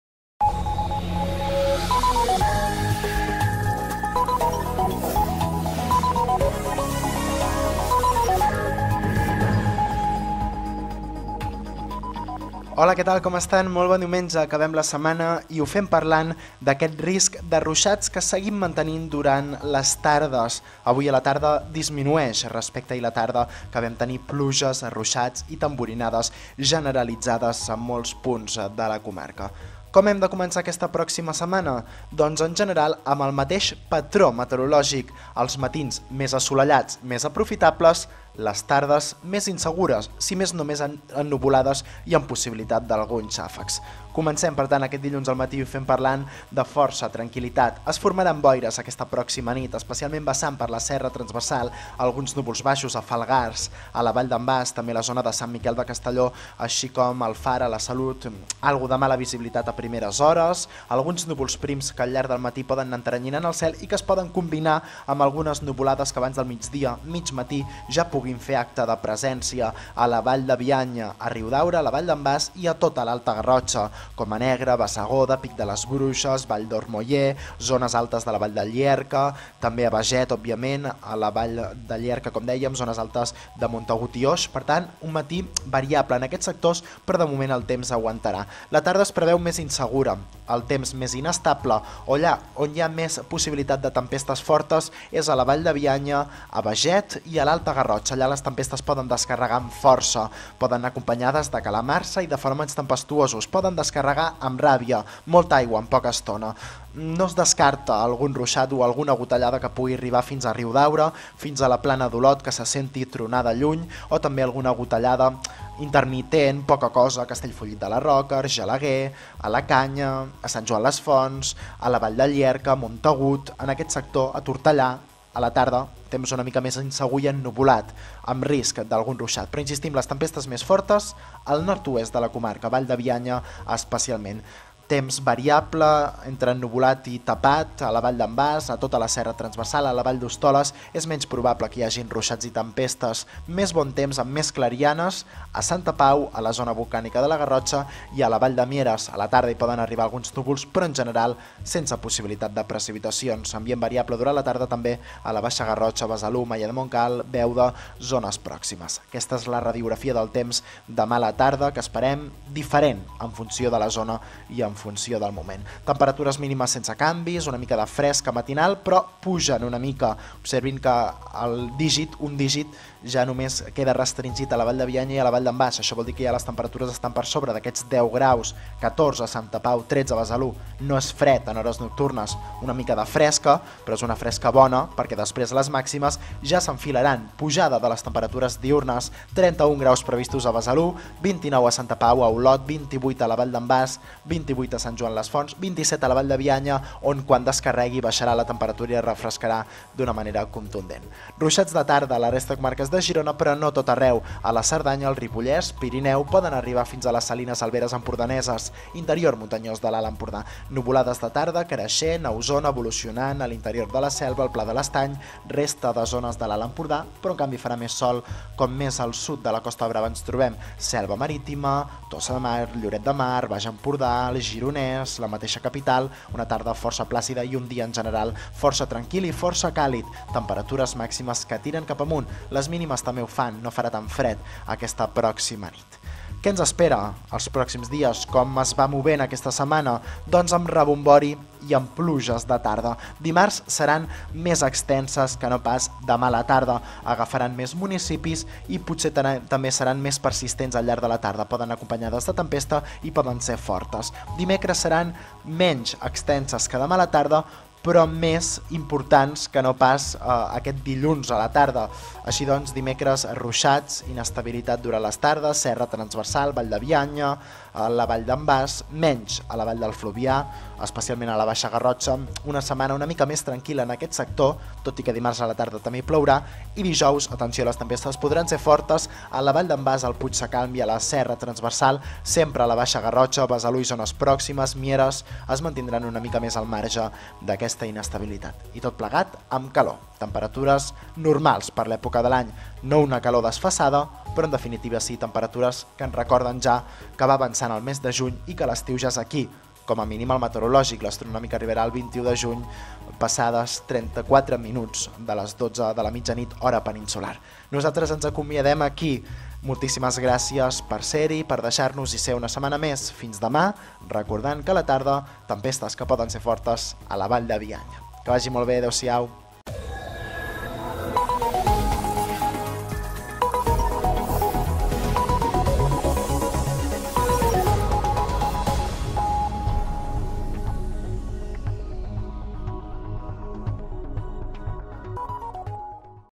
El temps diumenge 5 juny 2016
Risc de tempestes fortes de tarda a Bianya